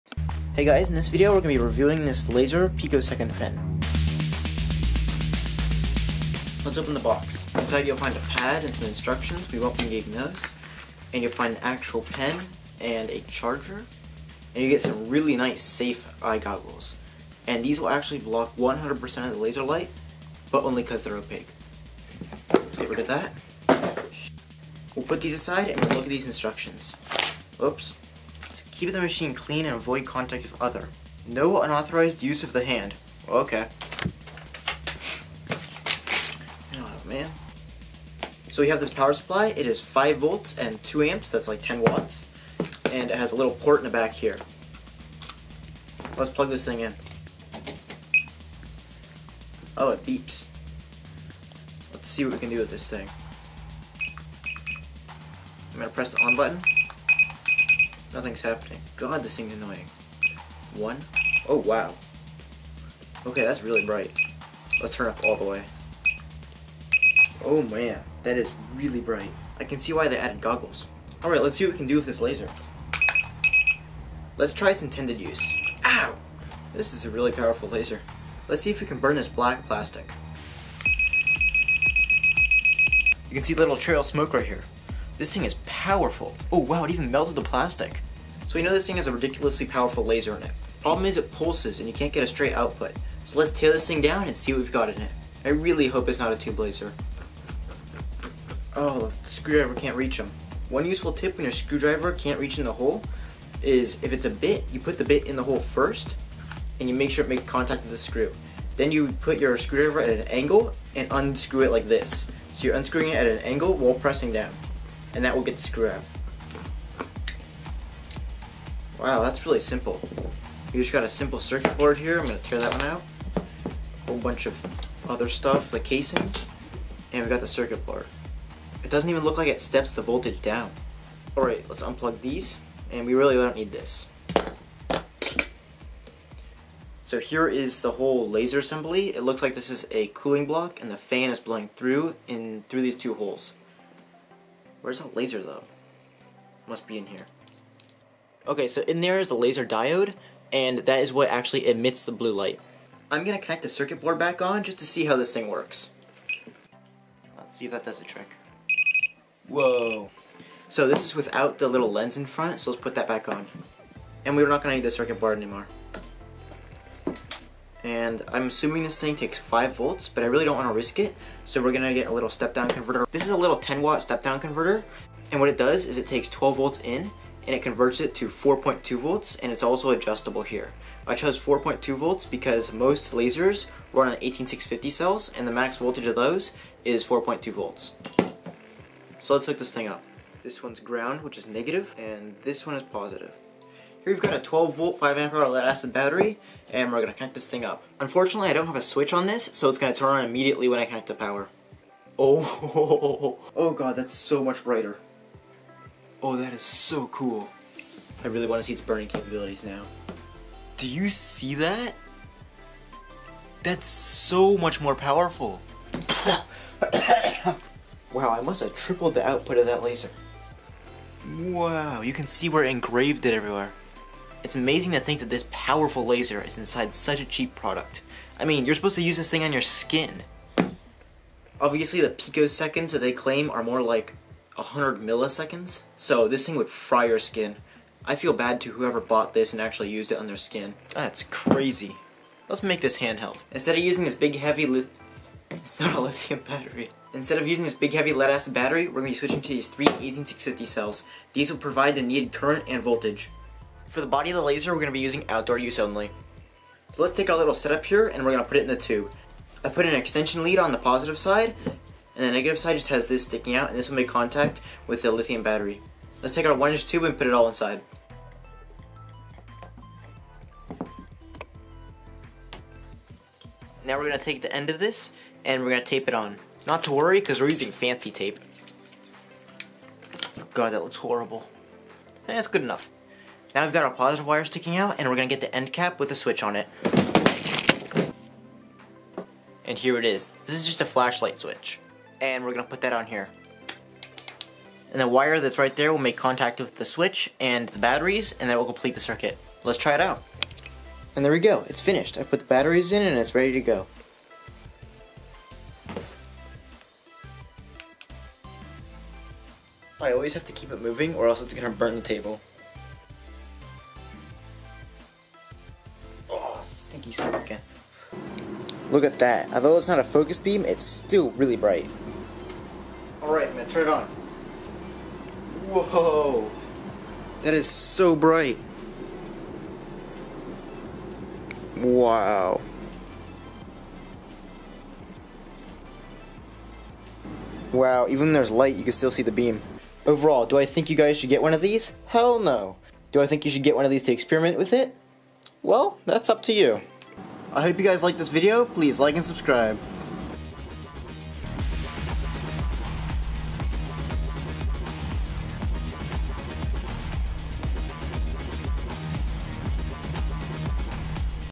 CREATING A POWERFUL BURNING LASER from 35$ skincare product
ive had this laser pen for awhile  so i opened it up and  repurposed the parts  to make a high power handheld laser this s nothing compared to what @styropyro has done but it was my first time experimenting with lasers

money spent on video: 50$ + time